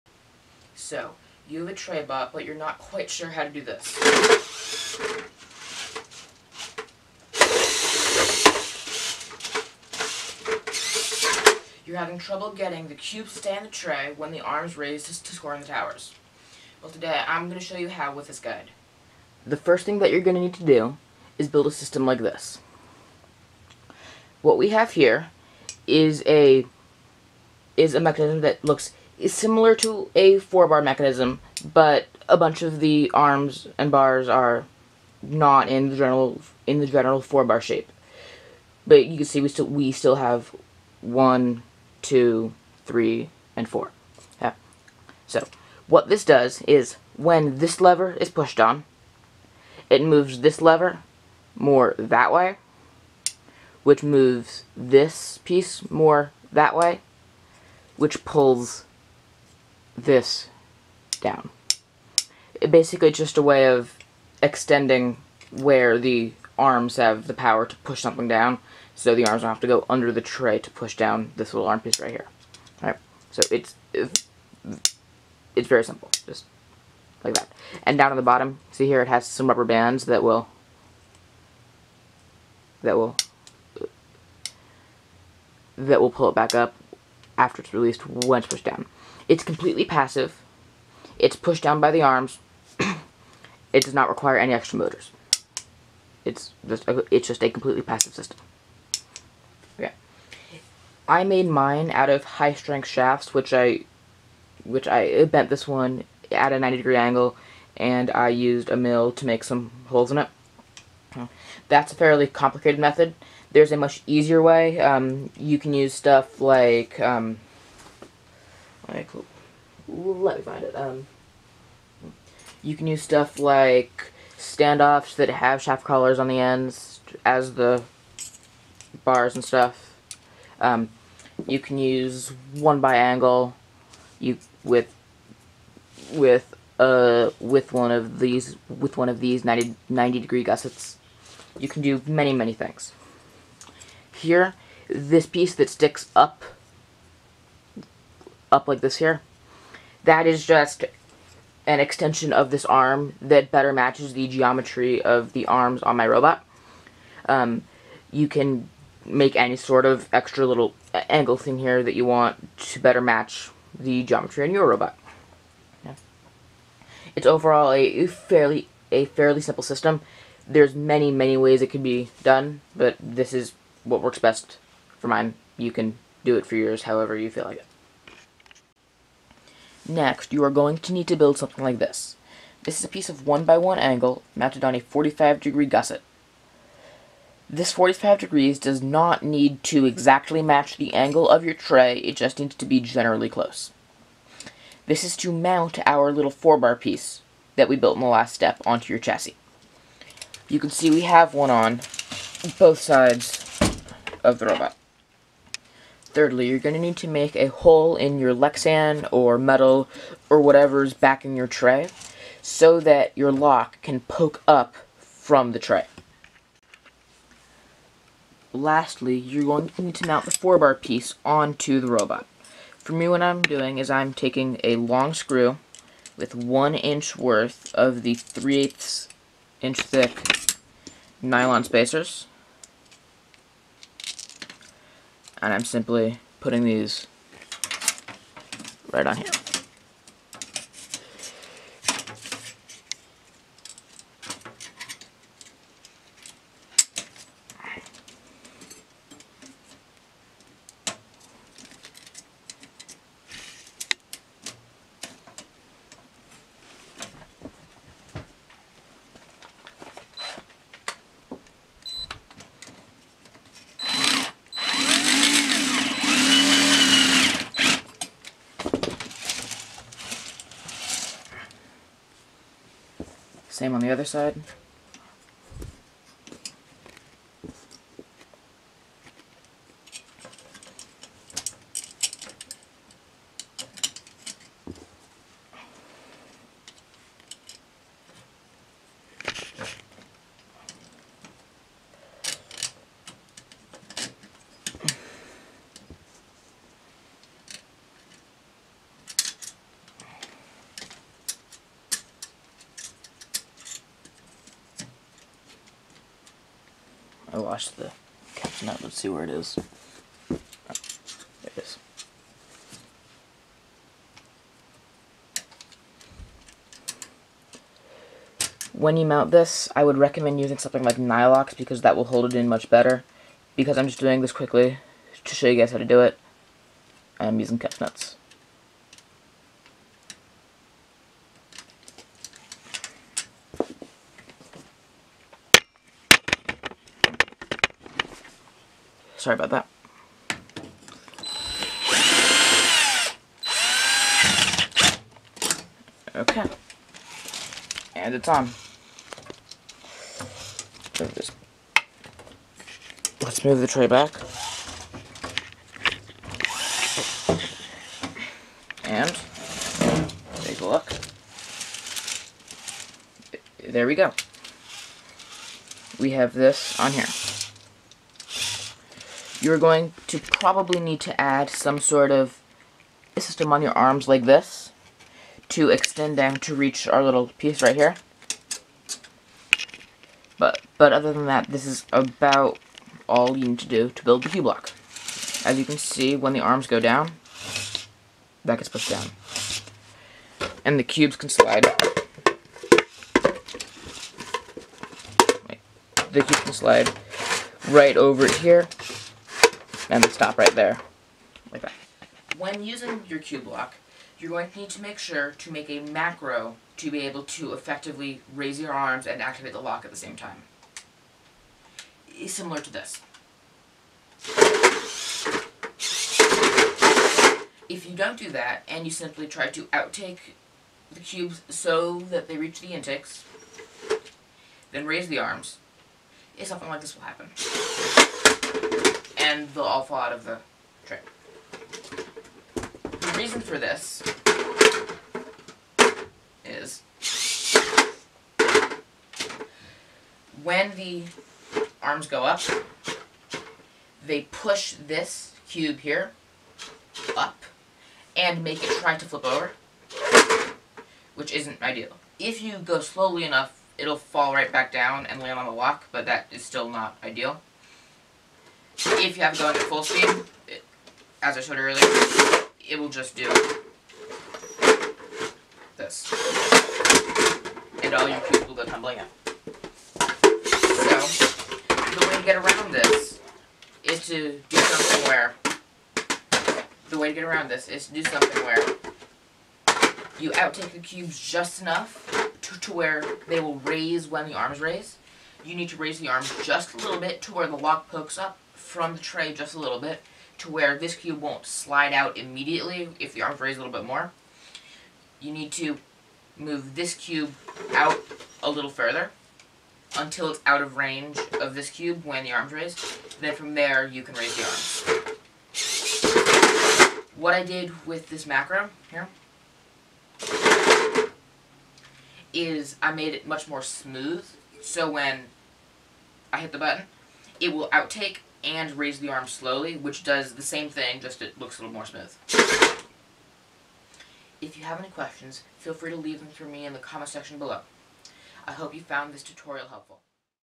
VEX Cube Lock Tutorial
This tutorial shows how to implement a cube lock onto a VEX Tower Takeover Traybot. 5090A, my sister team, gave me the inspiration to build this.